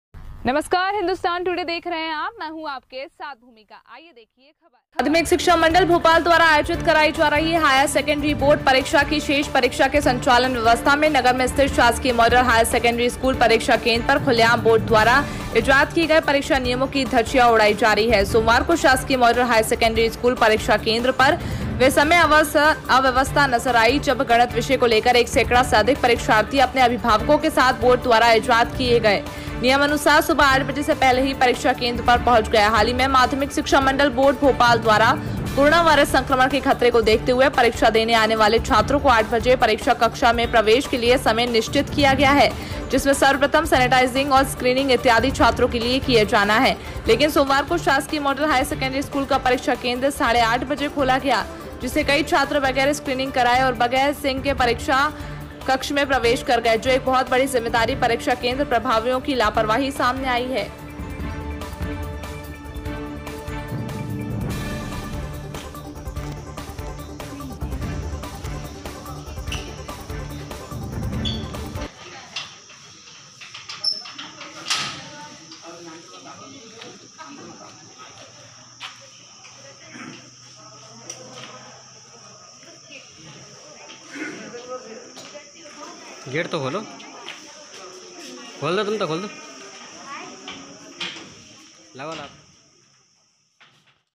परीक्षा को लेकर शासकीय मॉडल स्कूल पर जिम्मेदार उड़ा रहे हैं शासन के नियमों की धज्जियां
स्लग/परीक्षा को लेकर शासकीय मॉडल स्कूल पर जिम्मेदार उड़ा रहे हैं शासन के नियमों की धज्जियां/भितरवार/संवाददाता/कमल साहू

एंकर-- माध्यमिक शिक्षा मंडल भोपाल द्वारा आयोजित कराई जा रही हायर सेकेंडरी बोर्ड परीक्षा की शेष परीक्षा के संचालन व्यवस्था में नगर मैं स्थित शासकीय मॉडल हायर सेकेंडरी स्कूल परीक्षा केंद्र पर खुलेआम बोर्ड द्वारा इजाद किए गए परीक्षा नियमों की धज्जियां उड़ाई जा रही हैं।

सोमवार को शासकीय मॉडल हाई सेकेंडरी स्कूल परीक्षा केंद्र पर वह समय अव्यवस्था में नजर आई जब गणित विषय को लेकर एक सैकड़ा से अधिक परीक्षार्थी अपने अभिभावकों के साथ बोर्ड द्वारा इजाद किए गए नियम अनुसार सुबह 8:00 बजे से पहले ही परीक्षा केंद्र पर पहुंच गए। हाल ही में माध्यमिक शिक्षा मंडल बोर्ड भोपाल द्वारा कोरोना वायरस संक्रमण के खतरे को देखते हुए परीक्षा देने आने वाले छात्रों को 8:00 बजे परीक्षा कक्षा प्रवेश के लिए समय निश्चित किया गया है जिसमें सर्वप्रथम सैनिटाइजिंग और स्क्रीनिंग इत्यादि छात्रों की कि जाना है लेकिन सोमवार को शासकीय मॉडल हाई सेकेंडरी स्कूल का परीक्षा केंद्र 8:30 बजे खोला गया जिससे कई छात्र बगैर स्क्रीनिंग कराए और बगैर सिंह के परीक्षा कक्ष में प्रवेश कर गए जो एक बहुत बड़ी जिम्मेदार परीक्षा केंद्र प्रभारियों की लापरवाही सामने आई है